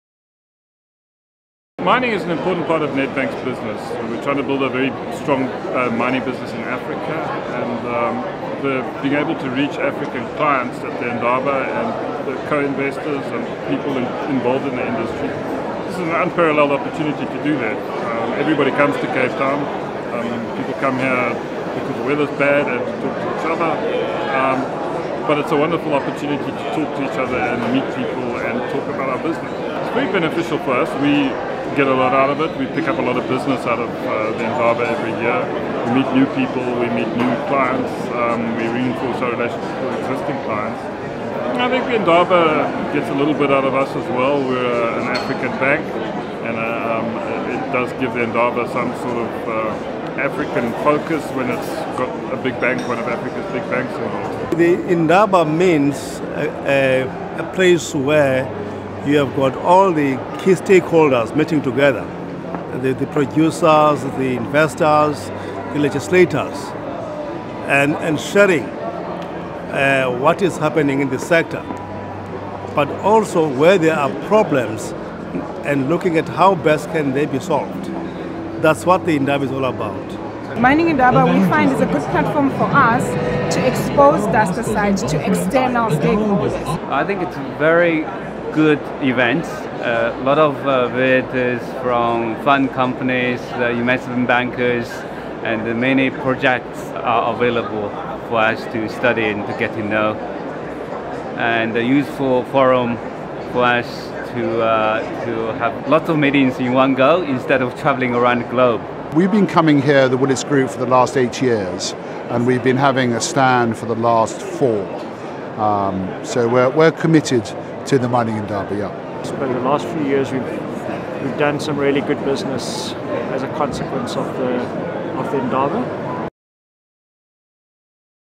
Mining Indaba 2014: Day 2 Testimonials of Mining Indaba
Testimonials from Day 2 of the 2014 Mining Indaba Conference. Testimonials from:

Mark Tyler, Nedbank Capital
Mzolisi Diliza, Strategic Partners Group
Lolly Chibi, Dust-A-Side
James Shen, Magindustries Corp
Andrew Wheeler, Willis Group
Thane Duffin, Lombard Insurance Company Limited